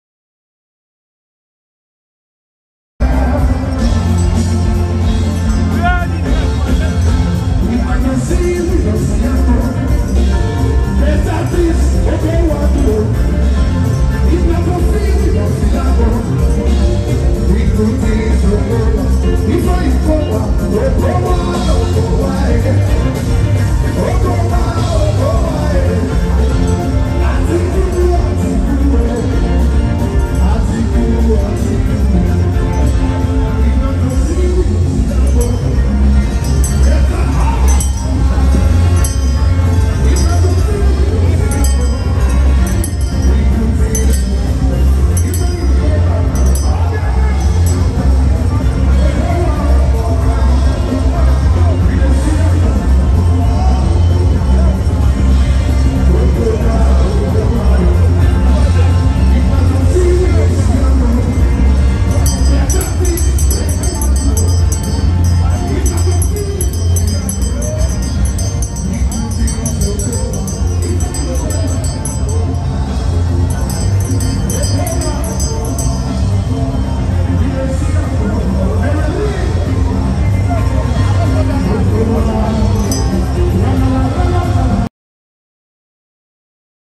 PDP PRESIDENTIAL CAMPAIGN RALLY IN ASABA. ATIKU CAMPAIGN IN DELTA. ATIKU/OKOWA IN ASABA
The presidential campaign rally of the People's Democratic party PDP continues today in Asaba the capital of DELTA State. Everyone is happy to welcome Alhaji Atiku Abubakar to Asaba for the campaign rally in DELTA State today. The presidential campaign rally of the PDP in Delta, Asaba is expected to be massive as Okowa is from there.